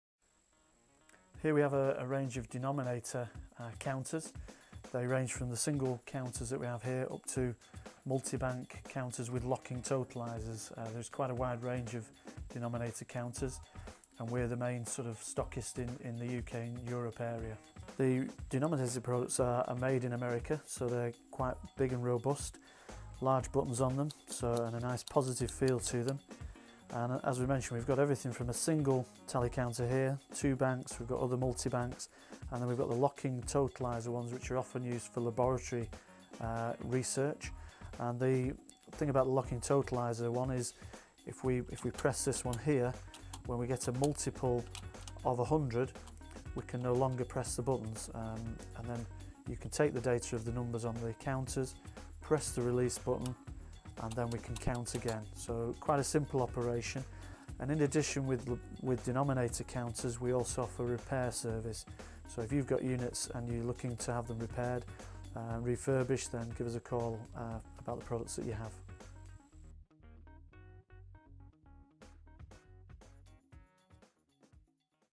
Denominator Counters Multi Bank tally Laboratory Counters
Starting from £49. Denominator analytic counters that helps you count any number of different things, such as analysis of blood cells in your Hematology or pathology laboratory. we Can also repair Denominators.